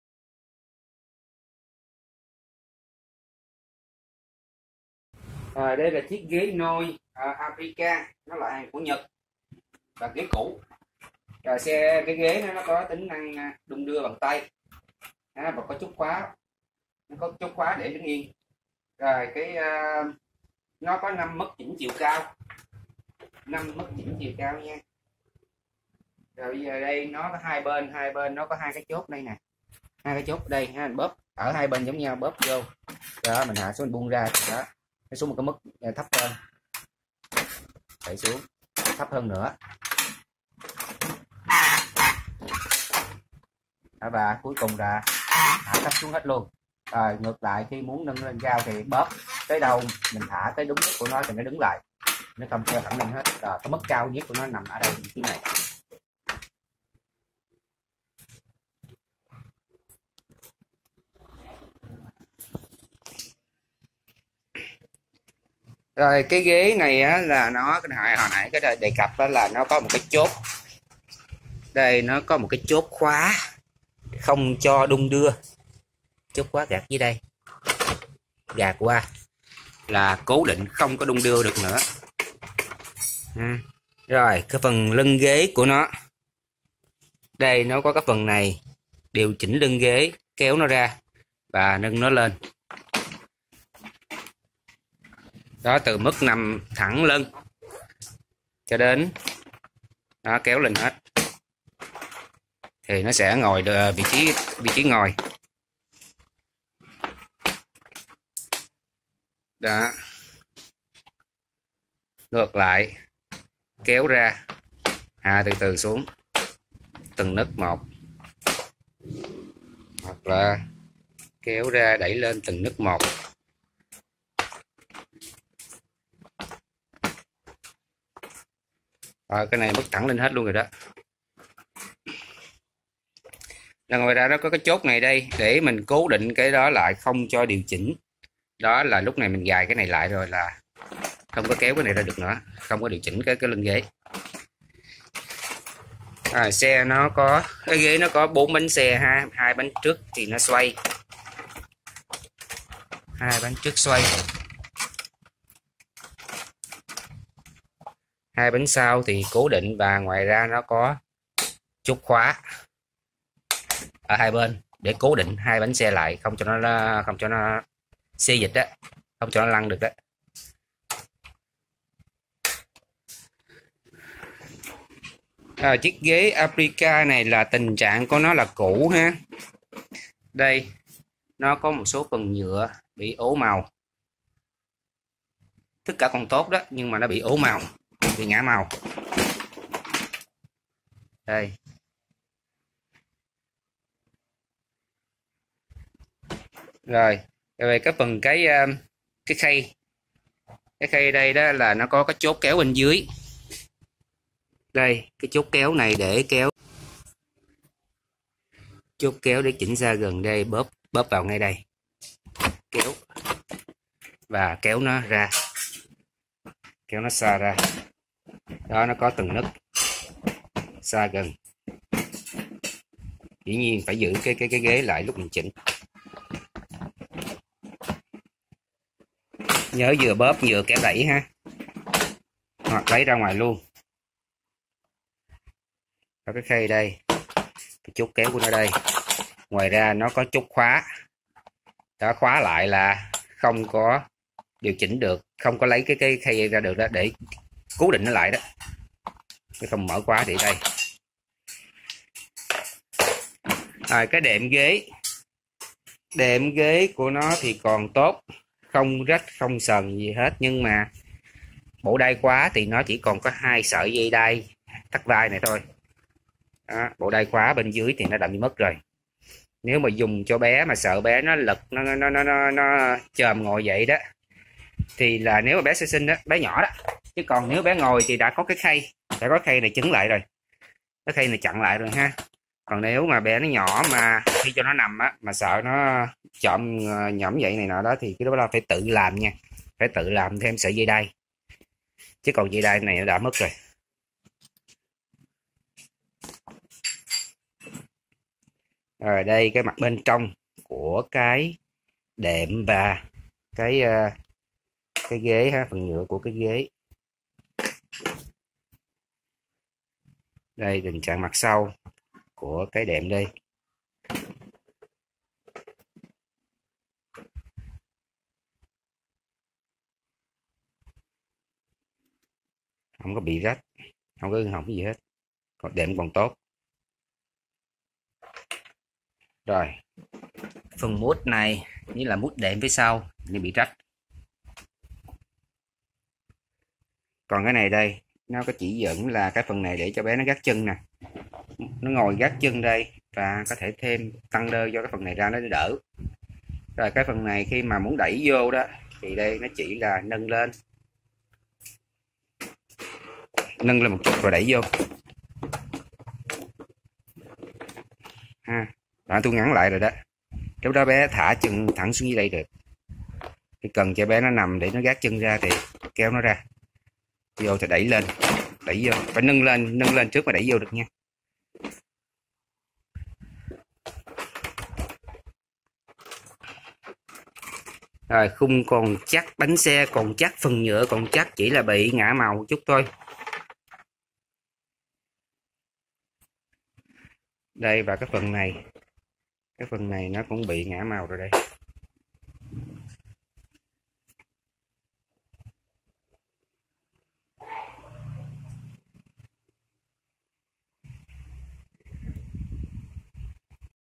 Ghế xe nôi – Ghế ăn dặm Aprica của NHẬT giá rẻ
HÀNG CŨ - Ghế xe nôi – Ghế ăn dặm Aprica của NHẬT
+ 5 Mức chỉnh chiều cao
+ có 4 bánh xe đẩy, 2 bánh trước xoay vòng, 2 bánh sau có khóa
+ lưng ghế điều chỉnh từ nằm thẳng lưng đến ngồi thẳng lưng
Tình trạng: HÀNG CŨ
* Vài chỗ nhựa bị ố vàng
* Không còn dây đai bảo vệ, khi dùng cho bé nhỏ nằm, nếu sợ bé lật hoặc nhổm ra khỏi ghế thì phải tự làm thêm dây đai bảo vệ
* Đệm,khung và tất cả mọi thứ còn lại đều còn tốt hết
Giá bán: 900.000
LH Shop Nguyên Phương - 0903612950
73/28 Huỳnh Văn Bánh P17 Q.Phú Nhuận TPHCM
KHÔNG NHẬN SHIP TỈNH